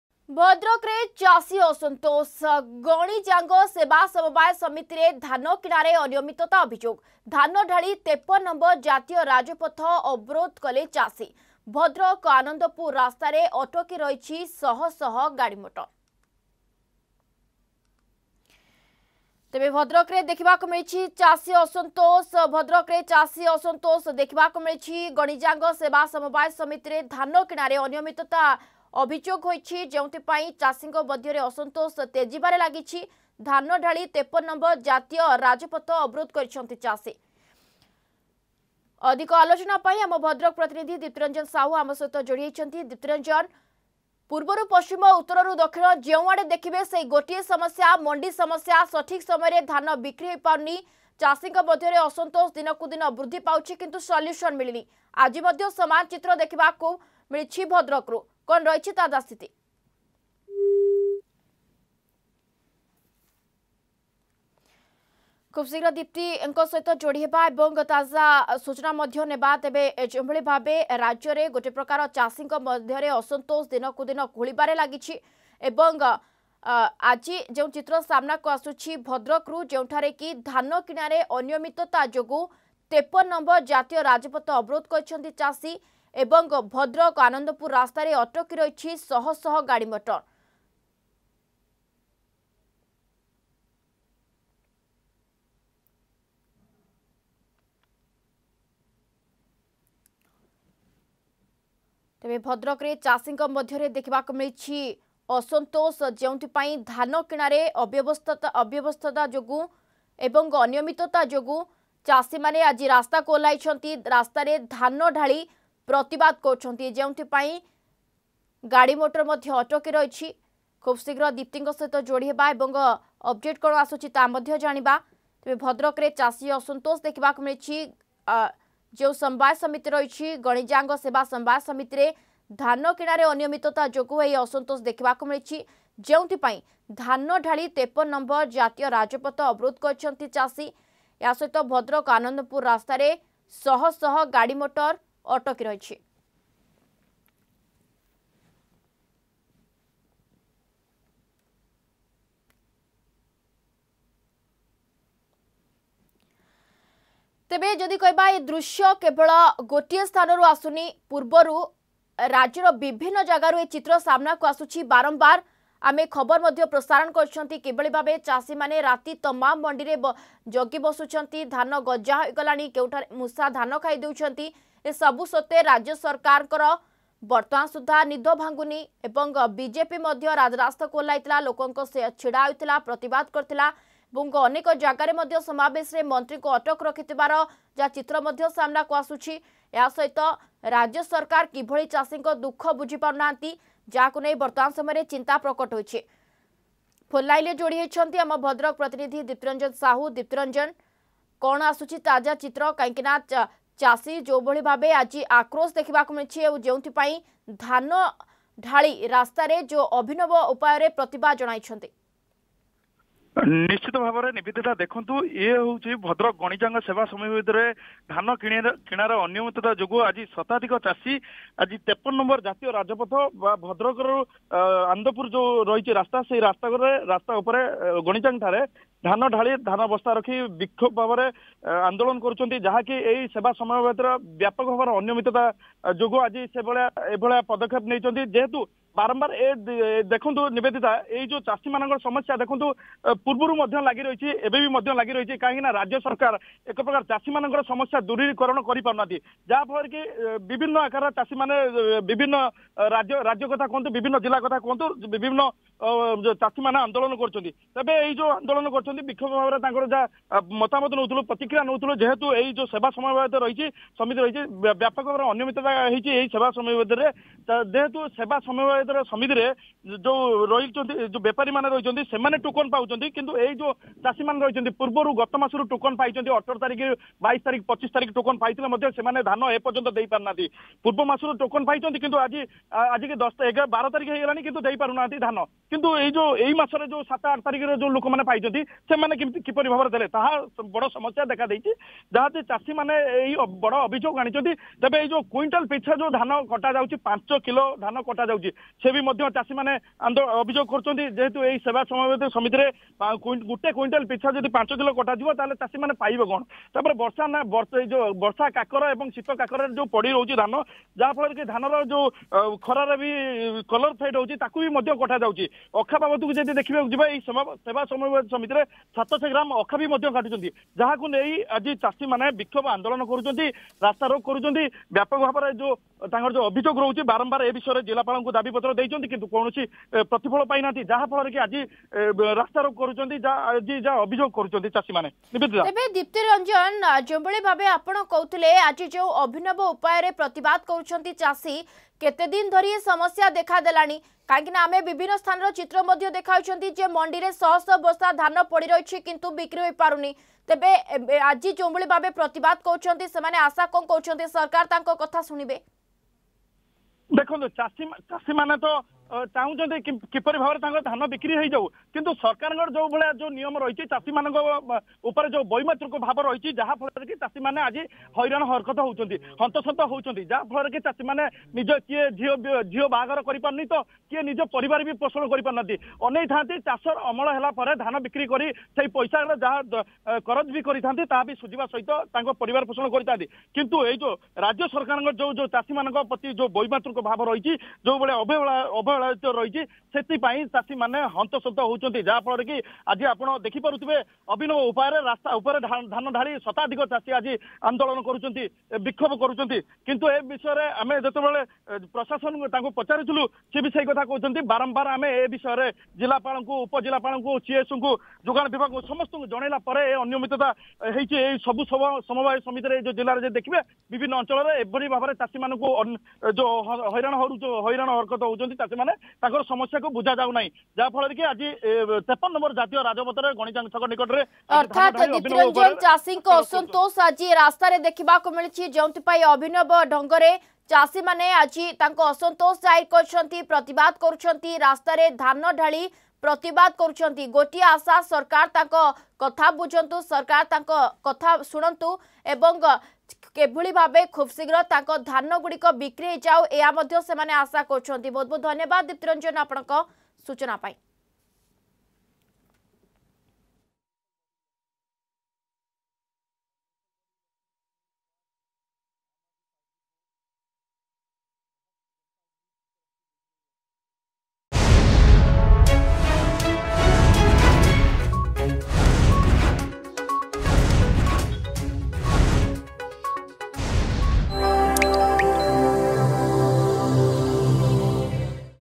ଭଦ୍ରକରେ ଚାଷୀ ଅସନ୍ତୋଷ ...
ଭଦ୍ରକରେ ଚାଷୀ ଅସନ୍ତୋଷ । ଗଣିଜାଙ୍ଗ ସେବା ସମବାୟ ସମିତିରେ ଧାନ କିଣାରେ ଅନିୟମିତତା ଅଭିଯୋଗ । ଧାନ ଢାଳି 53 ନଂ ଜାତୀୟ ରାଜପଥ ଅବରୋଧ କଲେ ଚାଷୀ । ଭଦ୍ରକ - ଆନନ୍ଦପୁର ରାସ୍ତାରେ ଅଟକି ରହିଛି ଶହ ଶହ ଗାଡି ମୋଟର .